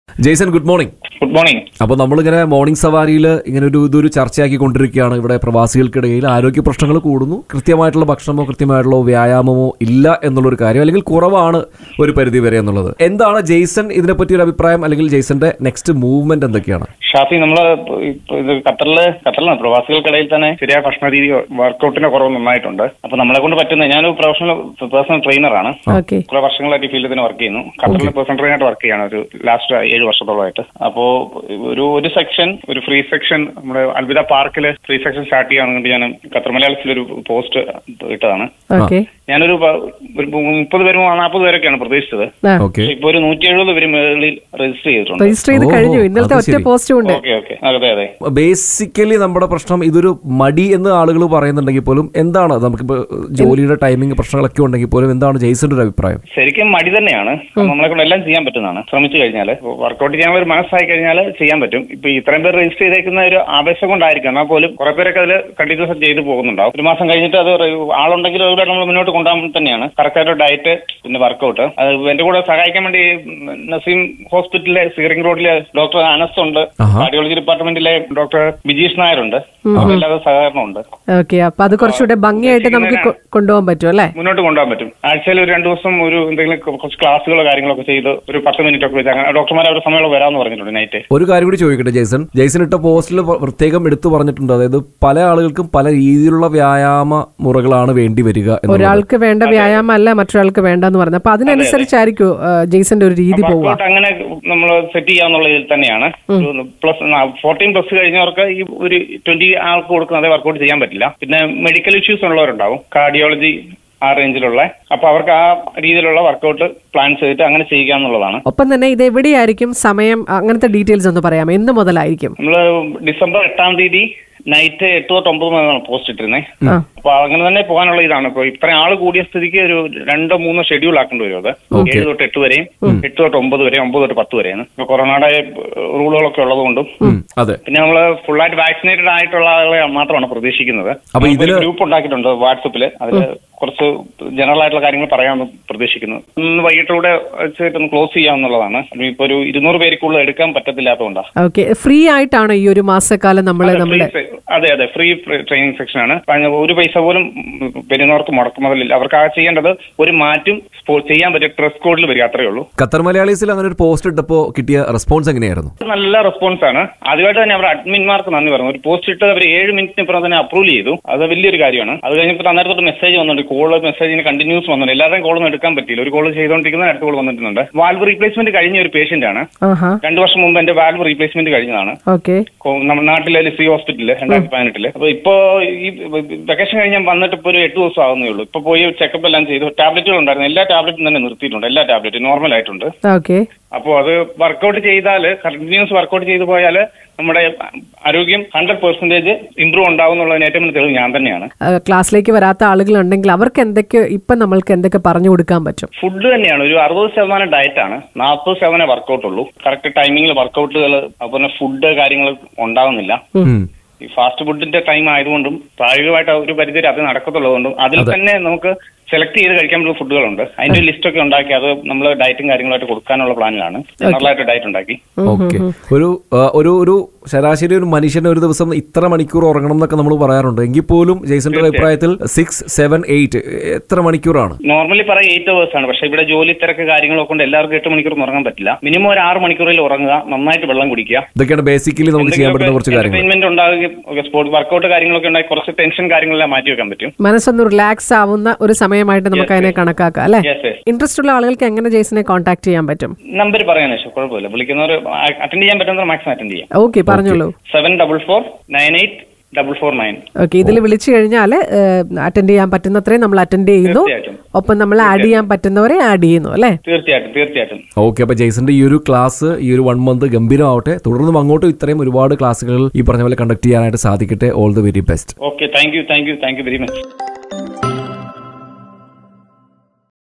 സൗജന്യ ഫിറ്റ്നസ് ട്രെയിനിങ് ഒരുക്കി ഖത്തർ പ്രവാസി ജെയ്സൺ | RADIO SUNO 91.7 FM
സൗജന്യ ഫിറ്റ്നസ് ട്രെയിനിങ് ഒരുക്കി ഖത്തർ പ്രവാസി ജെയ്സൺ